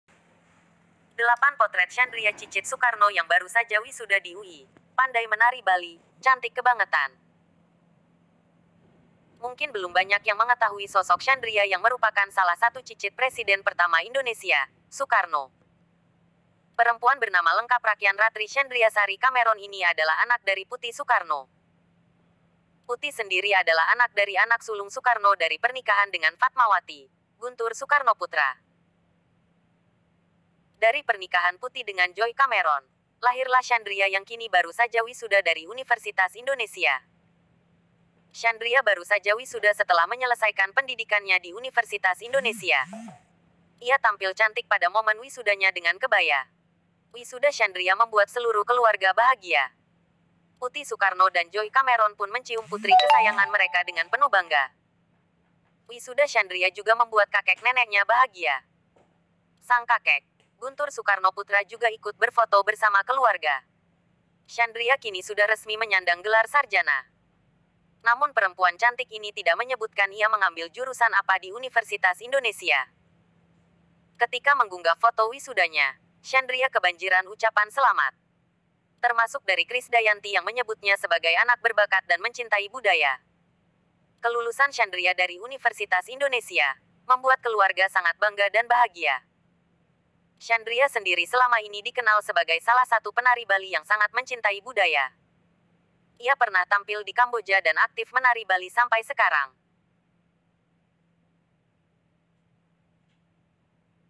Syandria Cicit Soekarno yang Baru Saja Wisuda di UI, Pandai Menari Bali - Cantik luar biasa
Mungkin belum banyak yang mengetahui sosok Syandria yang merupakan salah satu cicit Presiden pertama Indonesia, Soekarno. Perempuan bernama lengkap Rakyan Ratri Syandriasari Kameron ini adalah anak dari Puti Soekarno.

Puti sendiri adalah anak dari anak sulung Soekarno dari pernikahan dengan Fatmawati, Guntur Soekarnoputra. Dari pernikahan Puti dengan Joy Kameron, lahirlah Syandria yang kini baru saja wisuda dari Universitas Indonesia.

Syandria baru saja wisuda setelah menyelesaikan pendidikannya di Universitas Indonesia. Ia tampil cantik pada momen wisudanya dengan kebaya.

Wisuda Syandria membuat seluruh keluarga bahagia. Puti Soekarno dan Joy Kameron pun mencium putri kesayangan mereka dengan penuh bangga.

Wisuda Syandria juga membuat kakek neneknya bahagia. Sang kakek, Guntur Soekarnoputra juga ikut berfoto bersama keluarga.

Syandria kini sudah resmi menyandang gelar sarjana. Namun perempuan cantik ini tidak menyebutkan ia mengambil jurusan apa di Universitas Indonesia.

Ketika menggunggah foto wisudanya, Syandria kebanjiran ucapan selamat. Termasuk dari Krisdayanti yang menyebutnya sebagai anak berbakat dan mencintai budaya.

Kelulusan Syandria dari Universitas Indonesia, membuat keluarga sangat bangga dan bahagia.

Syandria sendiri selama ini dikenal sebagai salah satu penari Bali yang sangat mencintai budaya. Ia pernah tampil di Kamboja dan aktif menari Bali sampai sekarang.